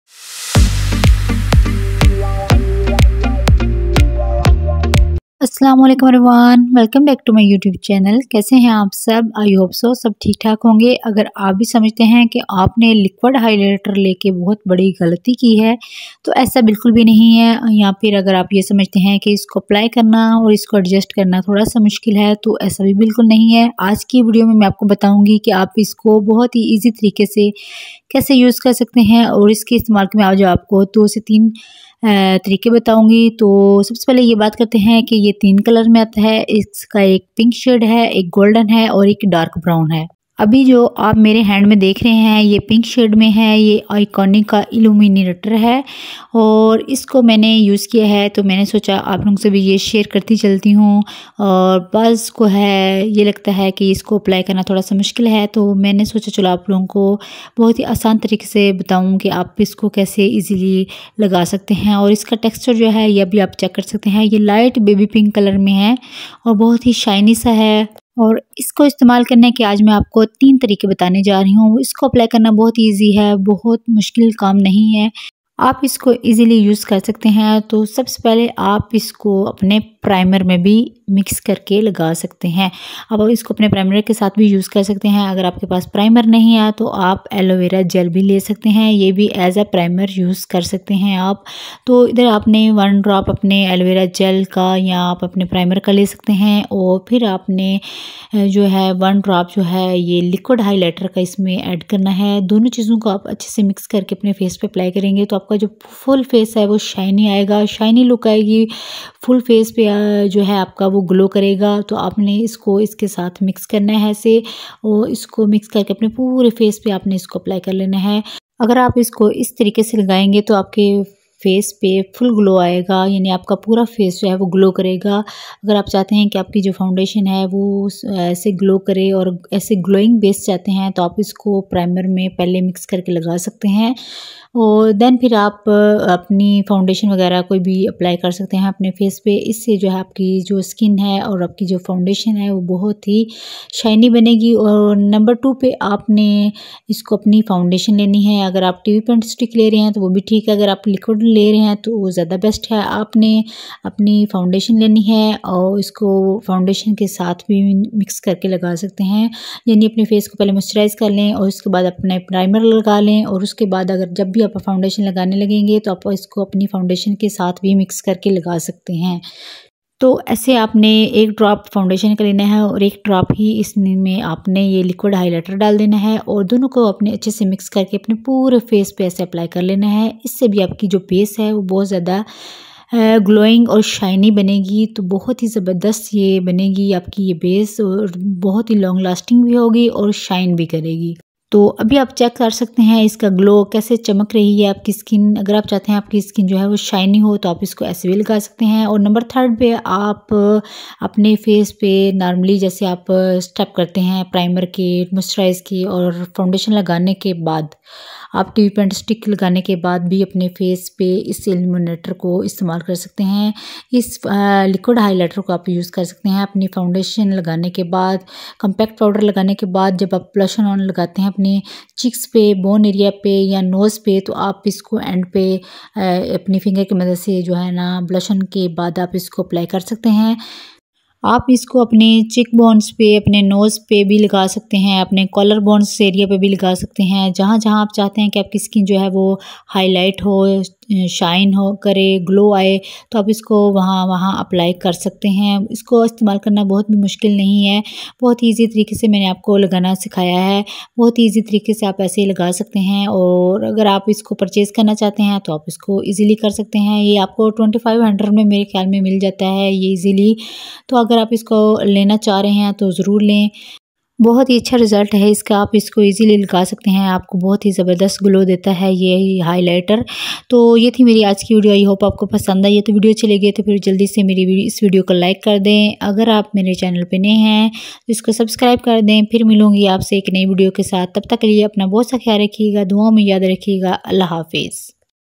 Extremely Affordable Iconic Illuminator|| 3 Best Way to Use liquid highlighter
Extremely Affordable Iconic Illuminator
3 Best ways to use liquid highlighter.
 Iconic London Illuminator honest review.
In this video I am sharing a honest review of iconic Illuminator.
How to use in easy way?
If you want to buy this liquid highlighter, you can buy it without any doubt.
This is so amazing highlighter. This highlighter give you a flawless makeup look.
This liquid highlighter is easy to use. you can easily apply on your face.
if you want to buy this highlighter, you can buy it without any doubt.